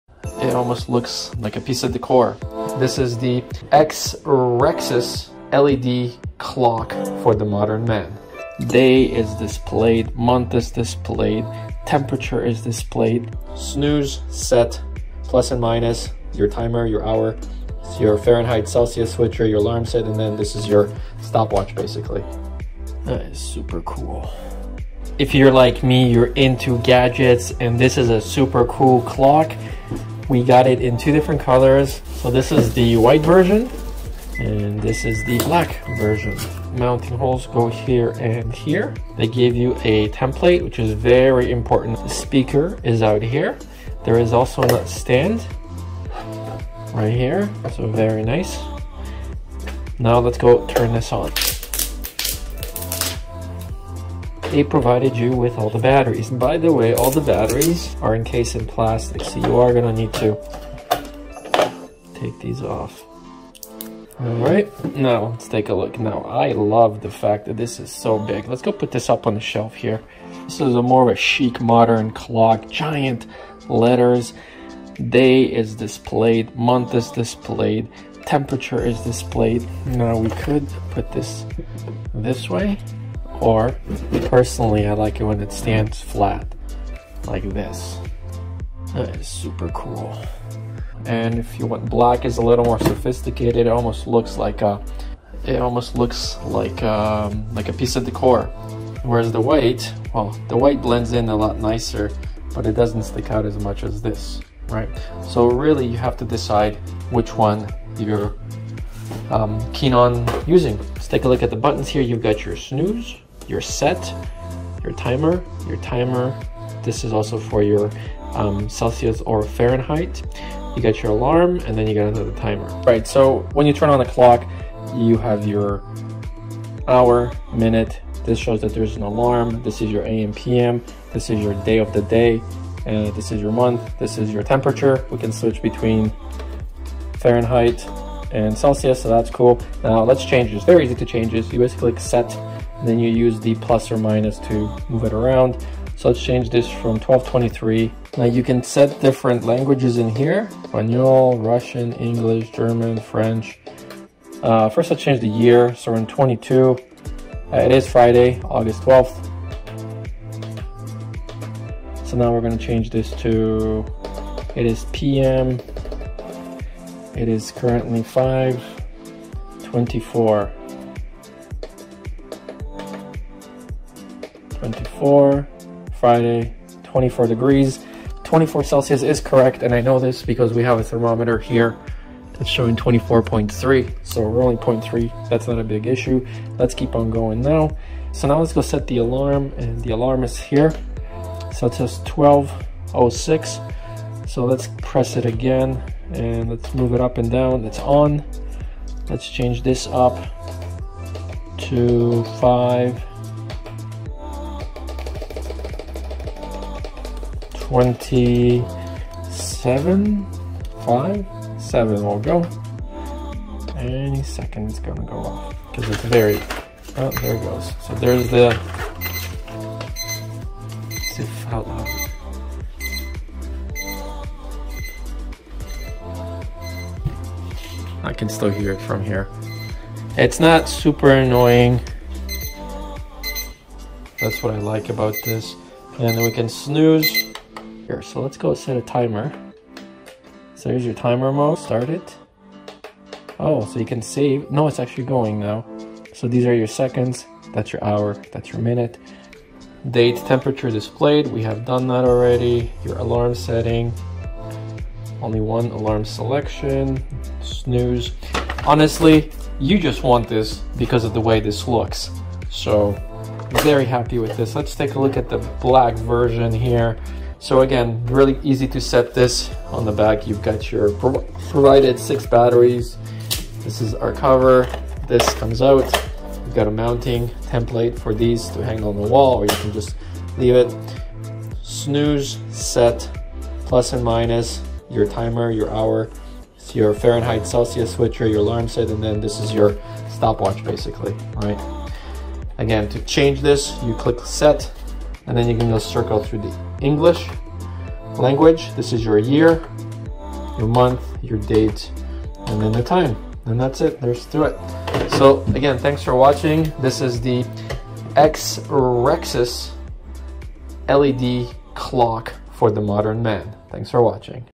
Cool Large Digital Clock - XREXS Large Digital Wall Clock Review with Temp/Timer/Alarms
- XREXS Large Digital Wall Clock for the modern home. With Electronic Alarm Clocks for Bedroom, great addition to any home decor, there is a Count Up & Down Timer. It has 14.17 Inch Large LCD Screen with Time/Calendar/Temperature Display (Batteries Included). Or get the Jumbo X Large clock at almost 17" screen.

XREXS Large 14" Screen Clock white
XREXS Jumbo 17" Screen Clock
XREXS Large 14" Screen Clock Black
XREXS Large 17" Screen Clock with Remote